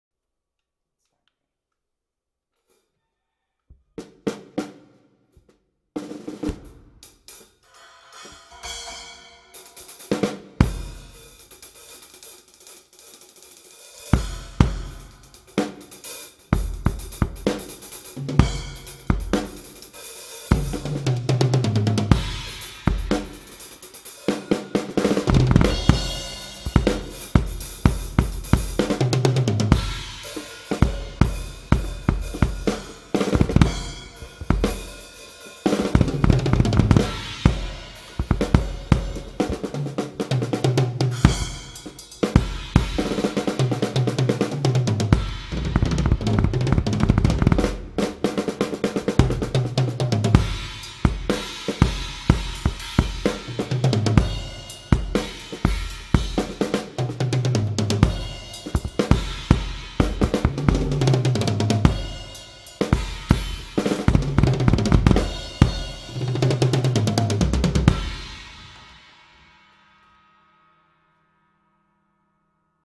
Yamaha EAD10 Test
I'm not a drummer and this is just a test for fun with my kid's drum without checking the owners manual. Awesome thing for having fun at home.